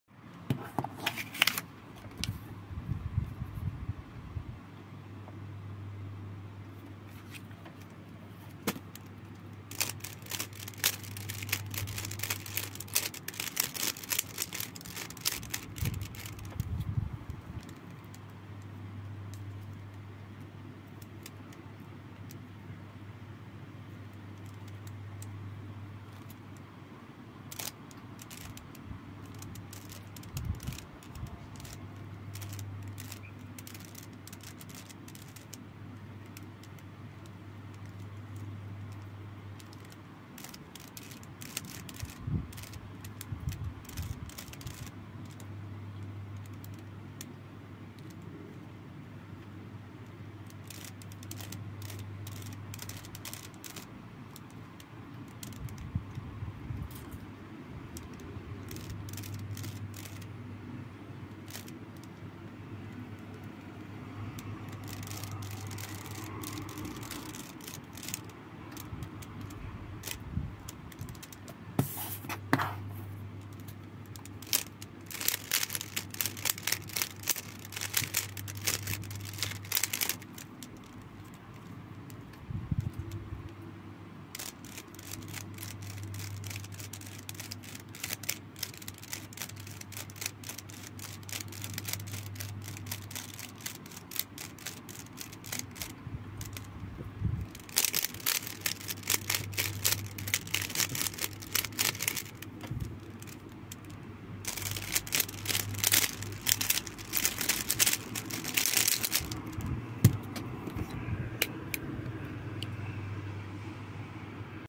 6.02 3x3 PB single!!!!!!!(recon)
Scramble: D2 R' D2 F2 D' L U' B' R2 L2 D2 F R2 B2 U2 L2 B D2 L F2
Not on stackmat tho
Sub-6 soon?